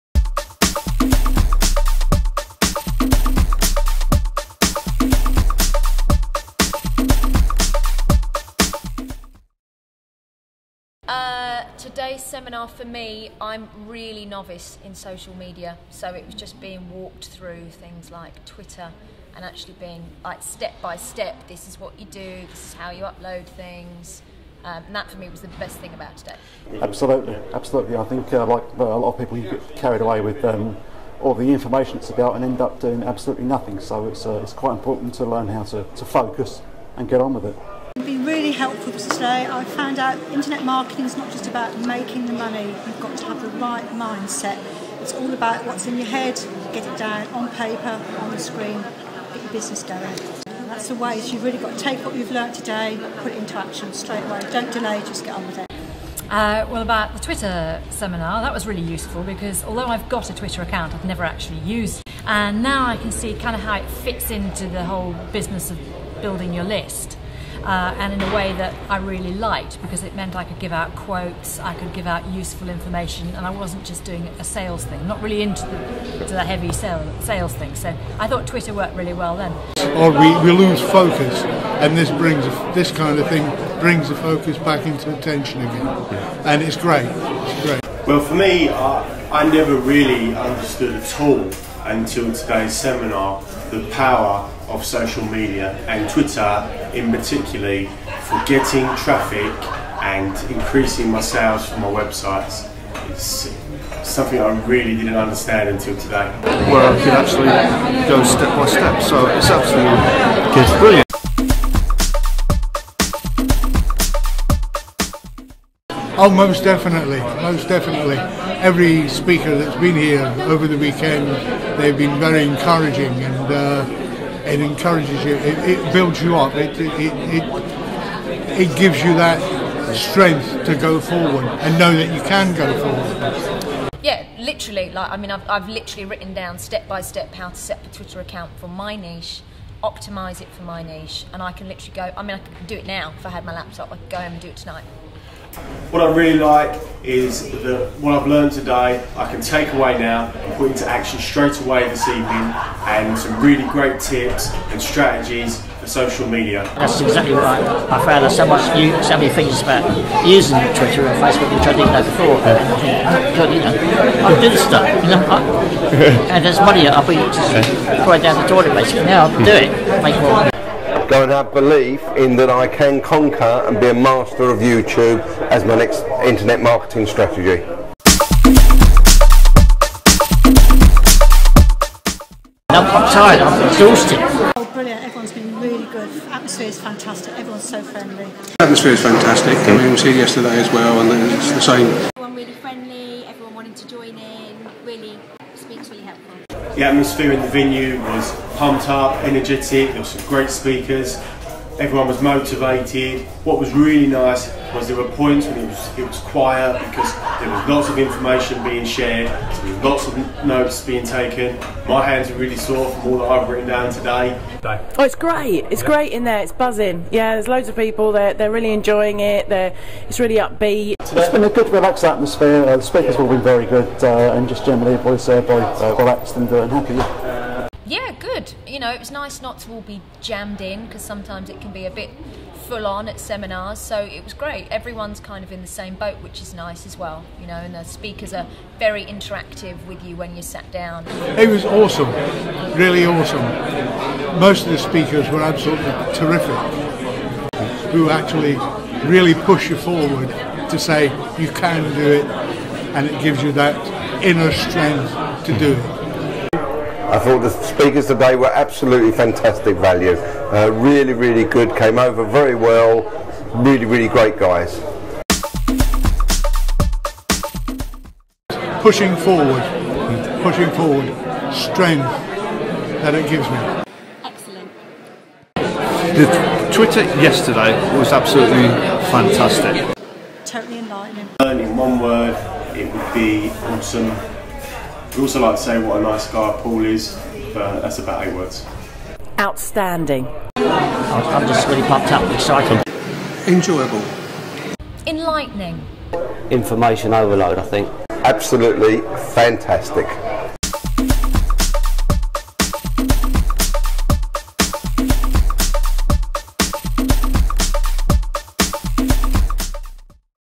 Social Media Event
Social Media experts Mili Ponce and Paul O'Mahony will train you on Social Media Marketing to be able to compete with the new challenges on online marketing.